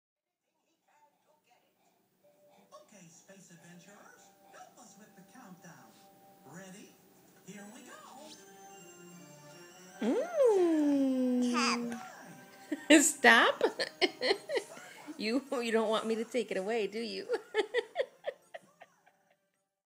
Coffee Lover
Faith is always trying to taste my coffee.  I never let her, but I let her smell it.   She's learned a new word... When I pull away my mug, she tells me to "stop."  Lol